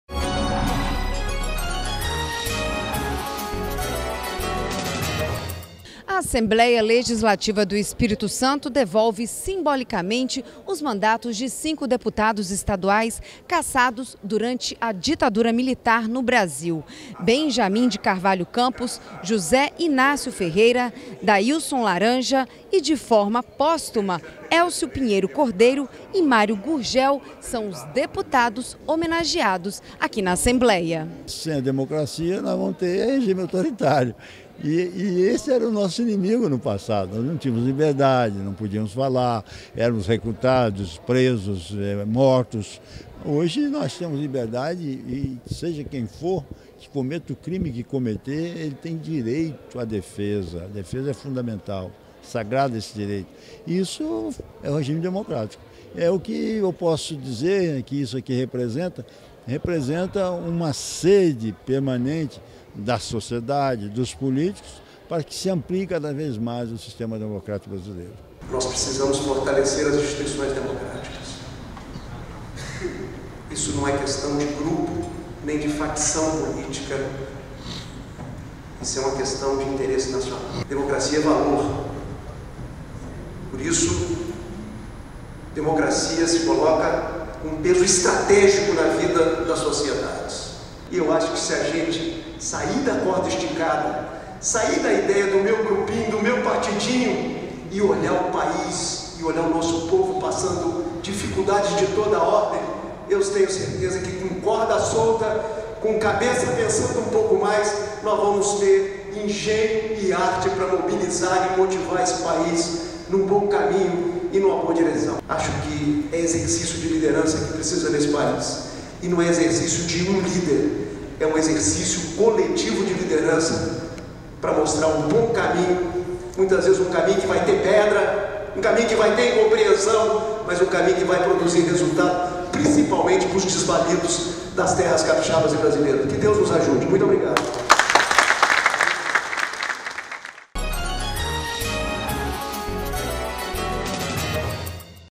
Paulo Hartung defende democracia em solenidade que devolve mandatos cassados no regime militar
O governador Paulo Hartung defendeu que as lideranças do país, independente das preferências políticas e ideológica, fortaleçam os valores democráticos e desempenhem o exercício coletivo de liderança para o país sair da maior recessão socioeconômica do Estado. O posicionamento de Hartung foi realizado, na tarde de terça-feira (12), durante Solenidade de Devolução Simbólica de Mandatos dos Deputados Estaduais cassados durante o período de Regime Militar. O evento foi realizado na Assembleia Legislativa, em Vitória.